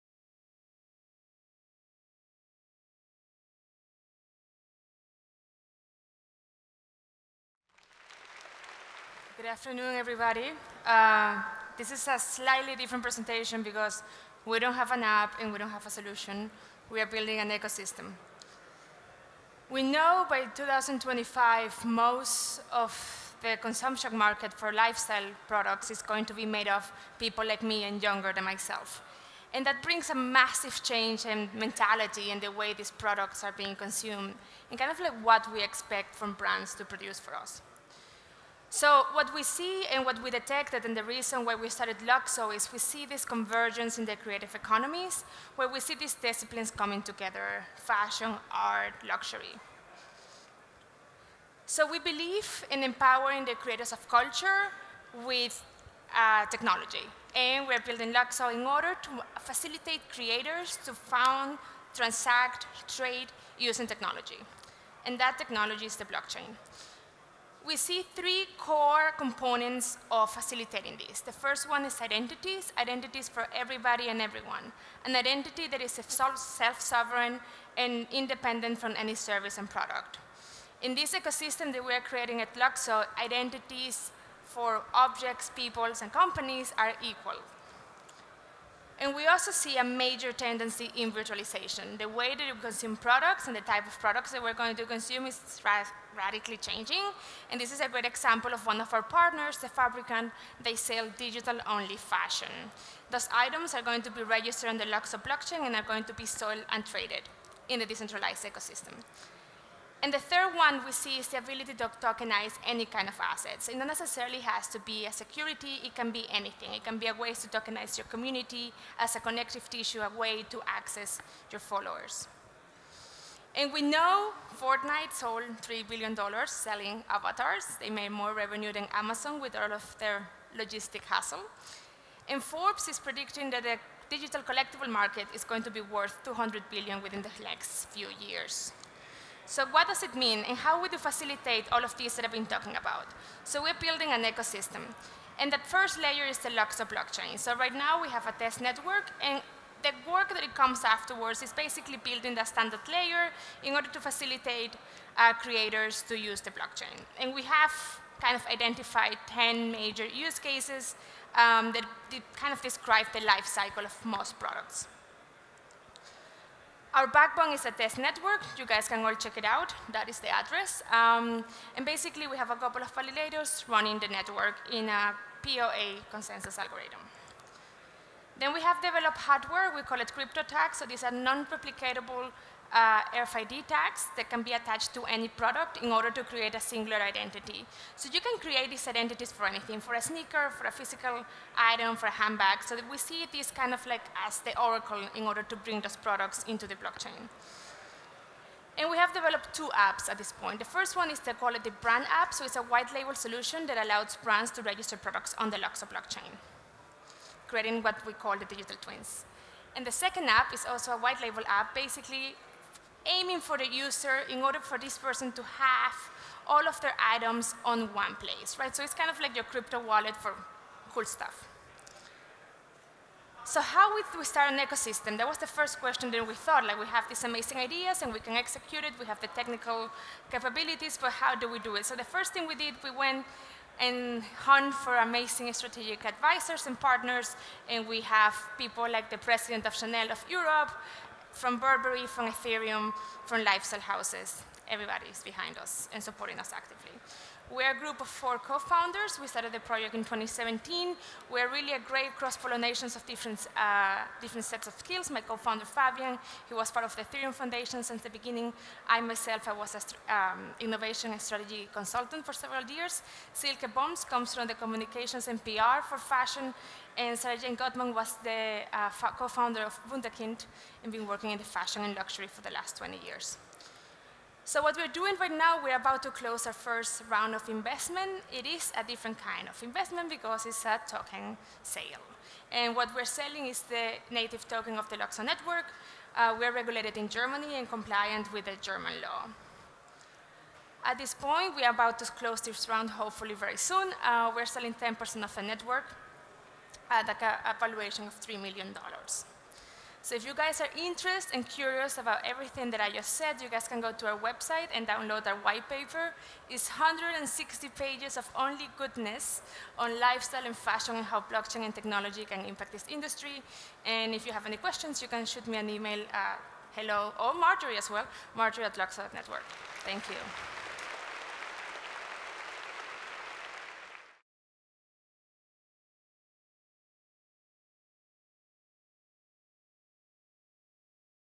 LUKSO - NOAH19 Berlin
Blockchain-Powered Businesses: Company presentation by Marjorie Hernandez de Vogelsteller, LUKSO at the NOAH Conference Berlin 2019, 13-14 June, STATION.